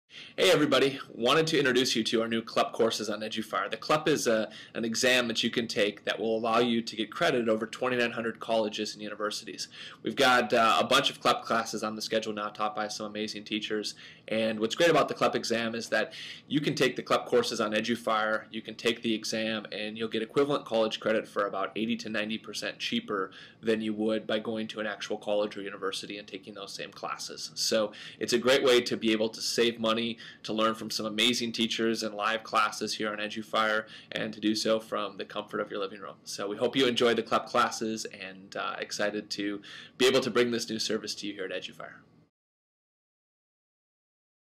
How to Pass the CLEP and Get College Credit on eduFire
Today eduFire is really excited to announce a set of six (and growing) CLEP Test Prep courses, aimed at helping you pass the CLEP so you can get college credit. The CLEP is a great way to skip the first one-to-two years of college, just by taking some $70 tests in various subjects. eduFire now offers courses in CLEP so you can learn the materials and get college credit from the comfort of your home.